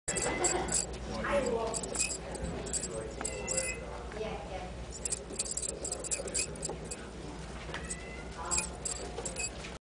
Meet PARTRIDGE a MOUSE-DOMESTIC currently available for adoption at Petango.com! 1/28/2011 12:46:04
Hi I'm here because my mom was pregnant.  She has a whole bunch of us.  We are all grown up now and moving out on our own.  We are here to looking for a great own with a great place for us to live out the rest of our lives.  Please consider adopting one of us today.